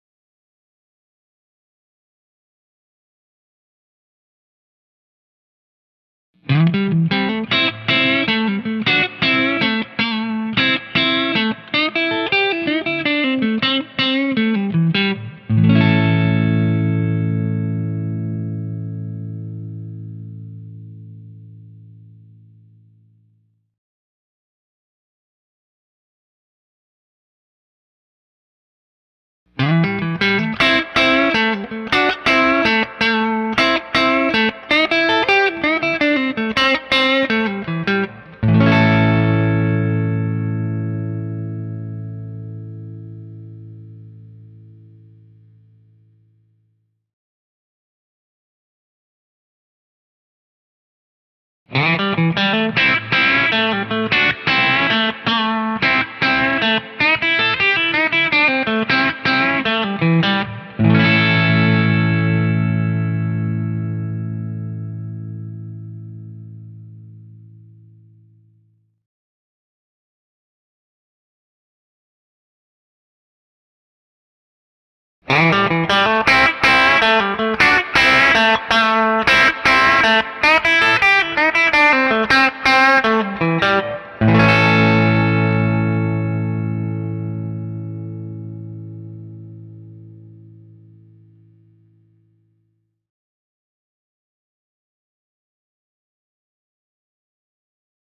Julius Music Box vs. Two Notes Torpedo Captor
Samples recorded using a Telecaster with a Fender Deluxe Reverb Amp into the Two Notes Torpedo Captor then again into the Julius Music Box. All amp settings remain the same. The IR loader used was the Mooer Radar Speaker Cab Simulator (simulating a 1960AG 4x12 Cab).